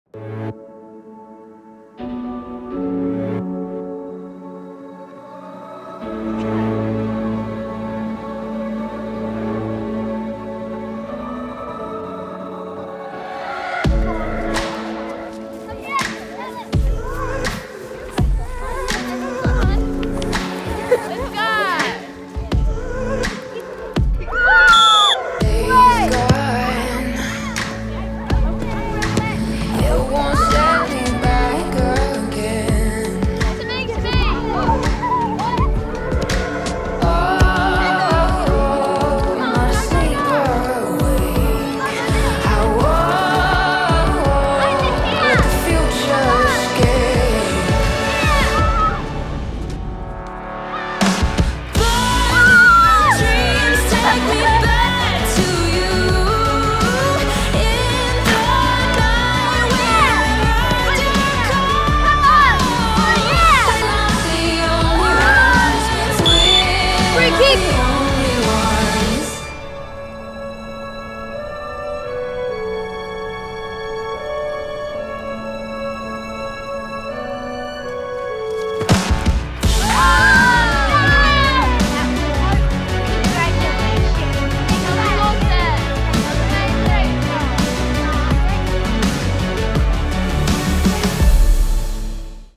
The Beautiful Game
Nestled amongst the Mackenzie tussocks underneath the jaw-dropping Aoraki Mt Cook, The Beautiful Game kicks off in the most beautiful place on earth, Aotearoa New Zealand ⚽️ ️